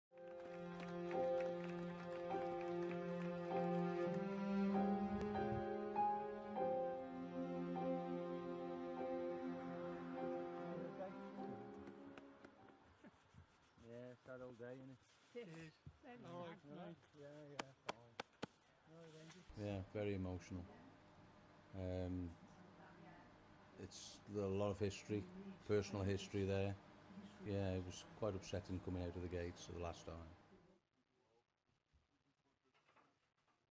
End of an era Production has finally ceased at Coalbrookdale
After more than 300 years, production has ceased at the Coalbrookdale ironworks in Shropshire as locals turn out to applaude as workers 'hang up their boots' for the final time.